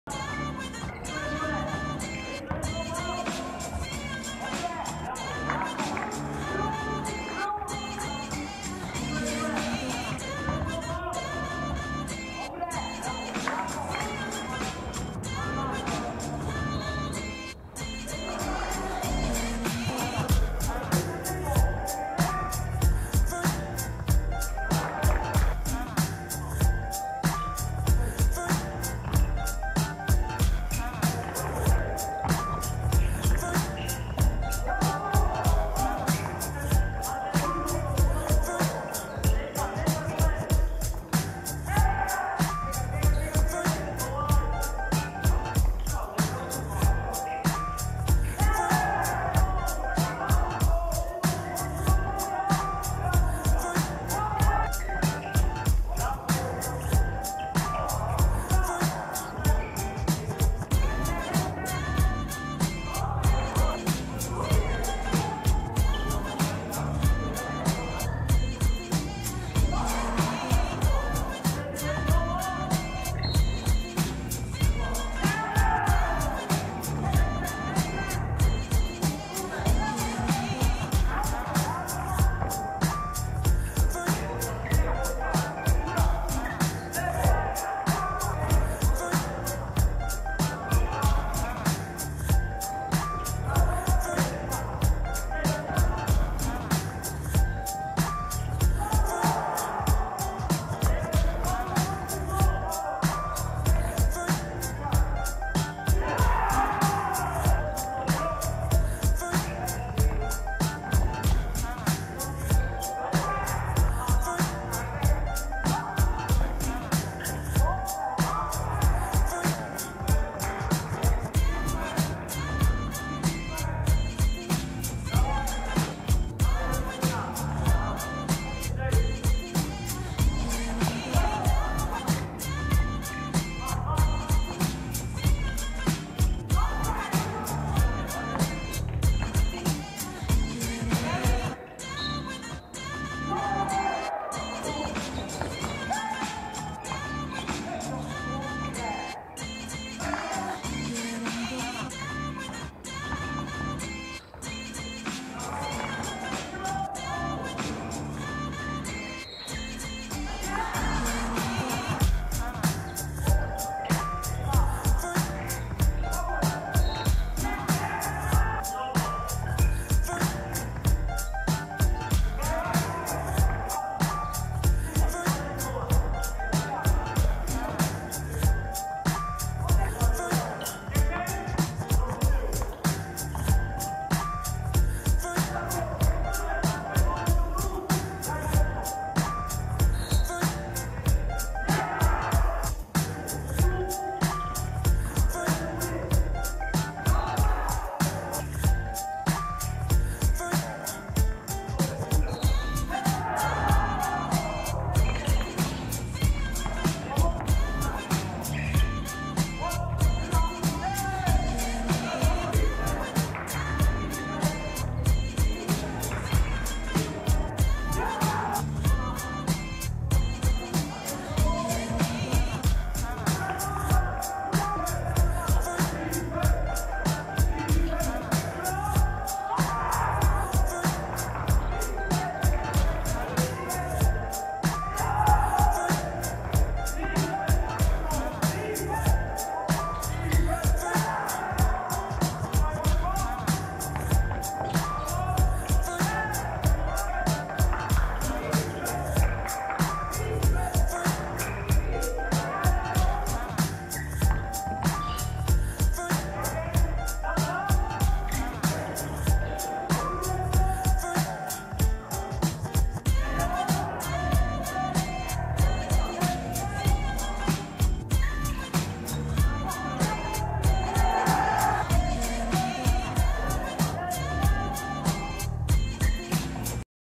03 08 Milwaukee vs Los Angeles – Draft Camp 31 Brodnica
MILWAUKEE 90:89 LOS ANGELES po dogrywce
W.Jaszczerski 31pkt 11zb
B.Młynarczyk 39pkt 7zb 3ast